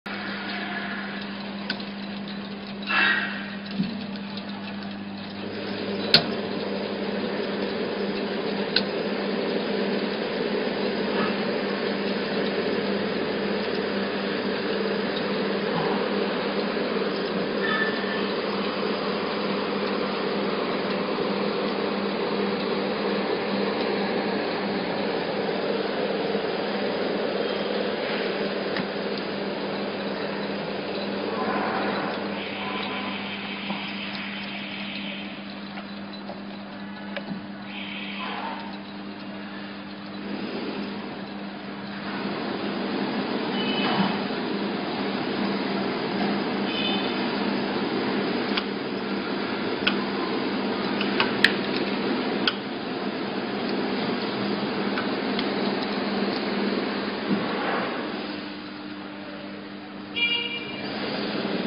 electric recycled waste paper color pencil making machines/shrink machine
How to make semi-automatic electric recycled waste paper color pencil making machines/shrink machine
1.Based on the purpose of protecting forest resources and ecological environment,saving the timber as well.
2.This recycles paper pencil machine developed by our company can utilize the recycled and useless newspapers,waste book papers etc to make pencils as the substitution for timber.
3.this kind of paper pencil is as hard as wooden pencil and very easy to be sharpened by the pencil sharpener.
4.Production capacity: 10,000—20,000pcs per day.
Technical parameter for electric recycled waste paper color pencil making machines
Cut paper→roll lead into paper→roll the paper into pencil with glue→dry the wet pencil→cut two sides of the pencil→polish the pencil→package the pencil with plastic film→fix the eraser on pencil.